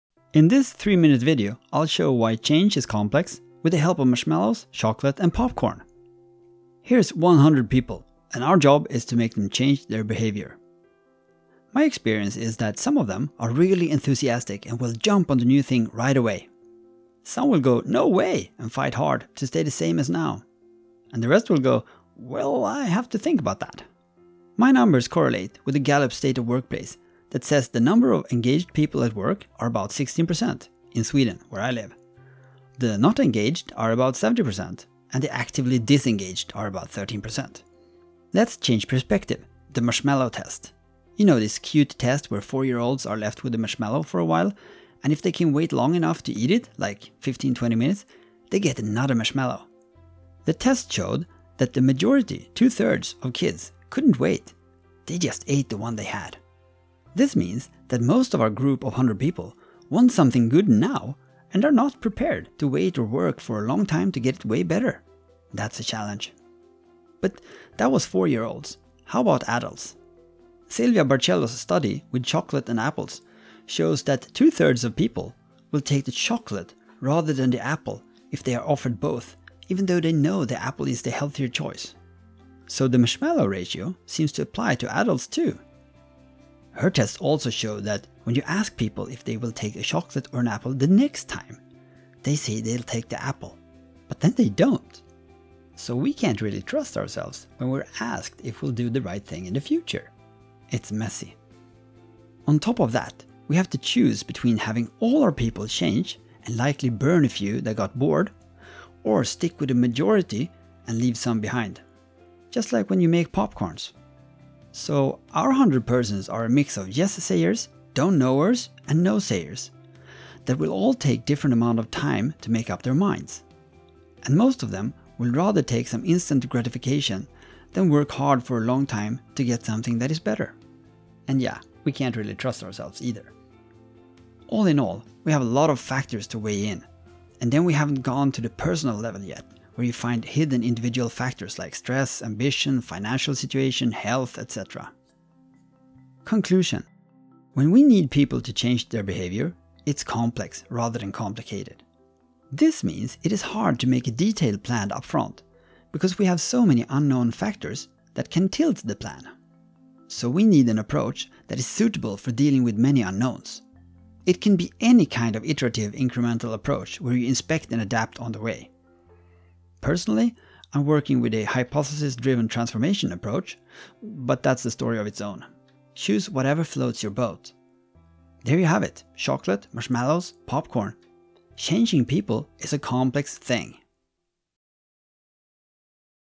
Chocolate, marshmallows and popcorn - changing people is complex
In this 3 minute video I'll show why changing people is complex, with the help of marshmallows, chocolate and popcorn…

Links to sources that are mentioned / referred to:
0:24 Engaged people - Gallup State of the global workplace
0:38 The Marshmallow test
1:09 Chocolate & apples: Silvia Barcello
1:42 Popcorn analogy by Svante Randlert (I think)
2:26 Complexity vs complicated as of Cynefin by Edward Snowden
2:49 Hypothesis driven transformation (in swedish)